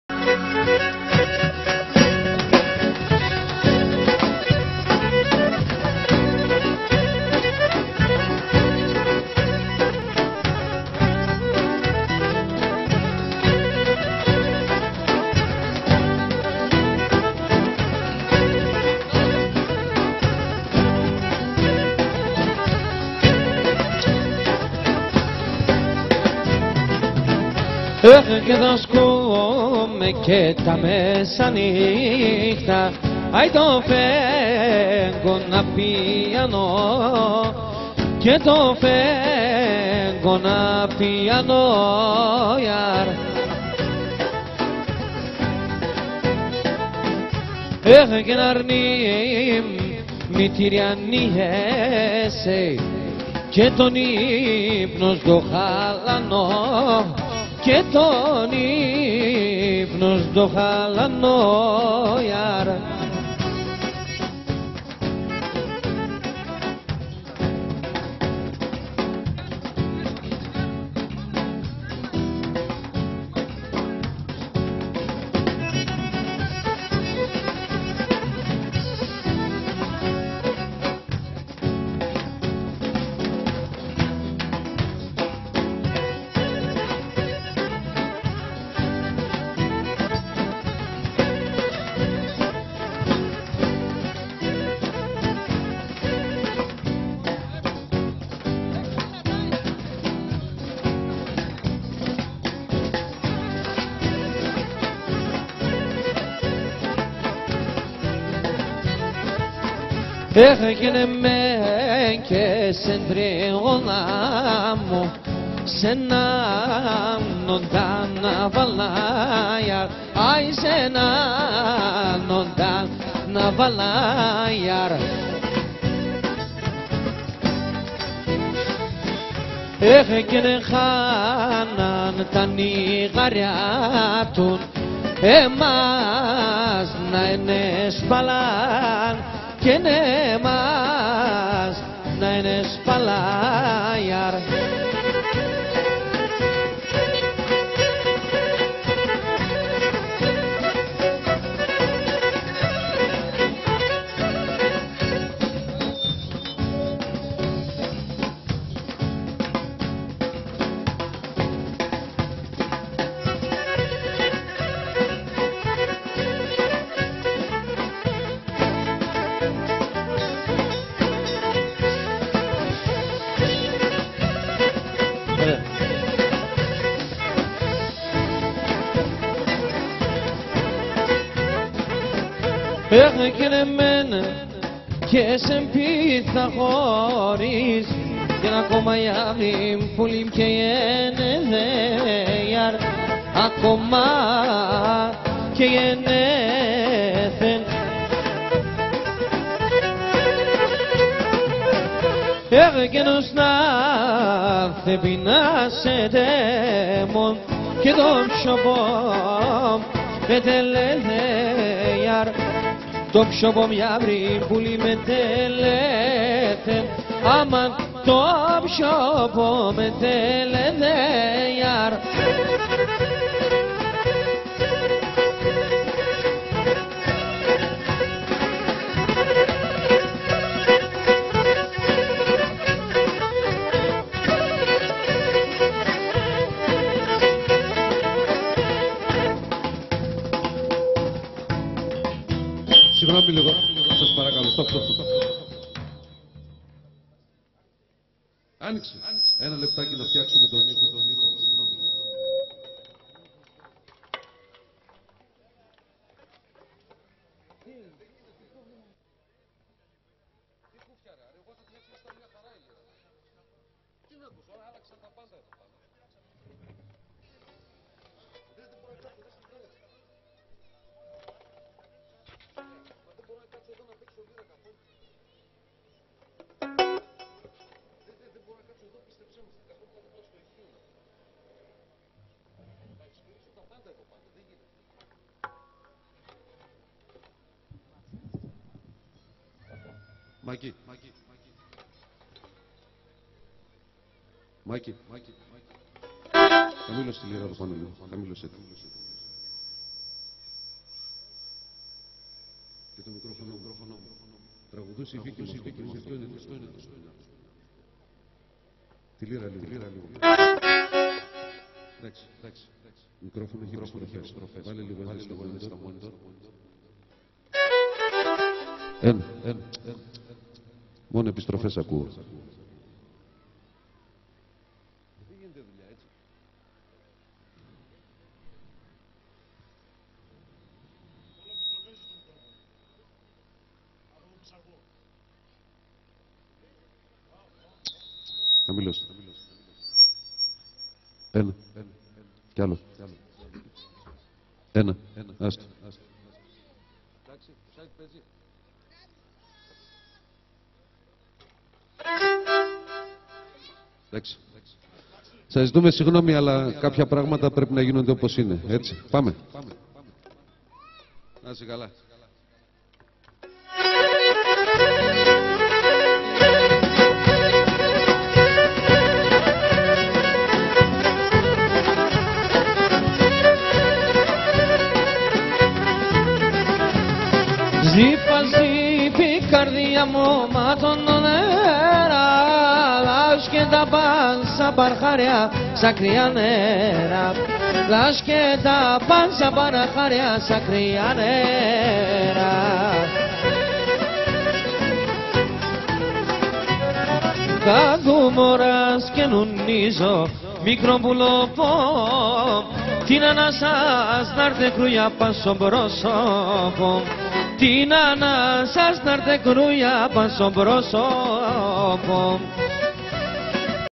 SIAMLIDIS KOTIDIS SKYDRA 2009 PART 5 PONTIAKA
Γιωργος Σιαμλιδης,Θοδωρος Κοτιδης,Γιαννης Παναγιωτιδης,Αρης και Βικυ Μιχαηλιδη στις εκδηλωσεις της Σκυδρας "ΣΤΑΥΡΕΤΚΑ 2009" στις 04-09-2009.Ηχητικη καλυψη Φαντιδης Λαζαρος